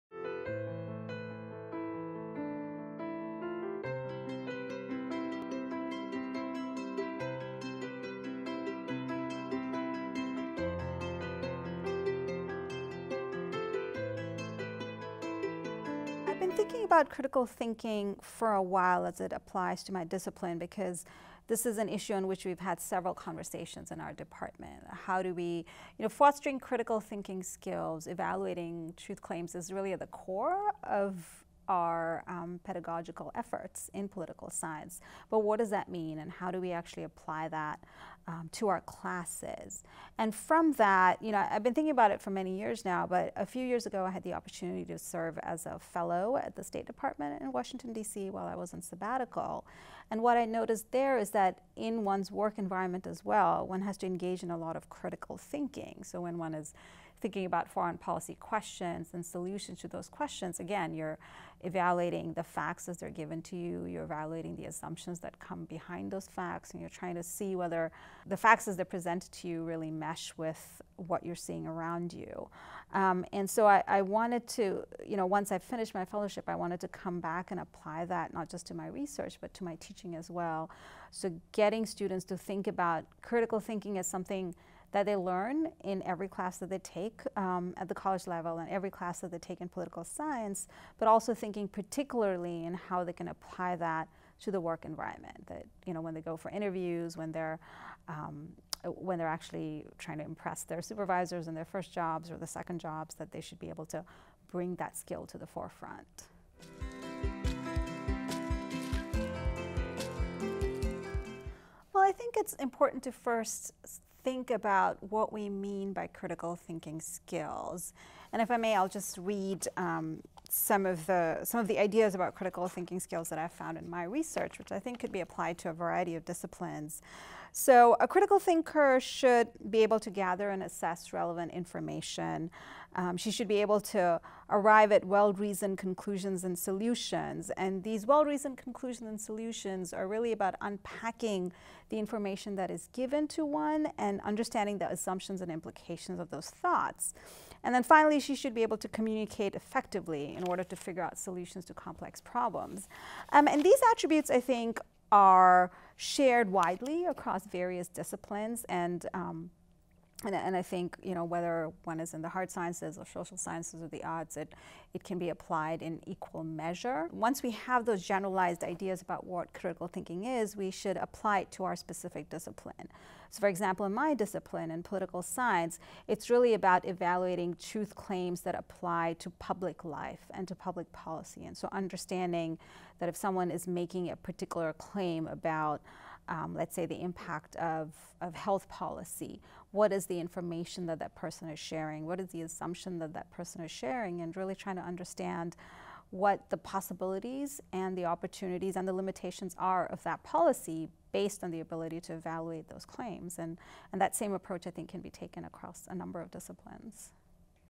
Culture of Critical Thinking - Bidisha Biswas, WWU (2 of 5)
Dr. Bidisha Biswas discusses how critical thinking has become part of the culture in political science as a skill for student success.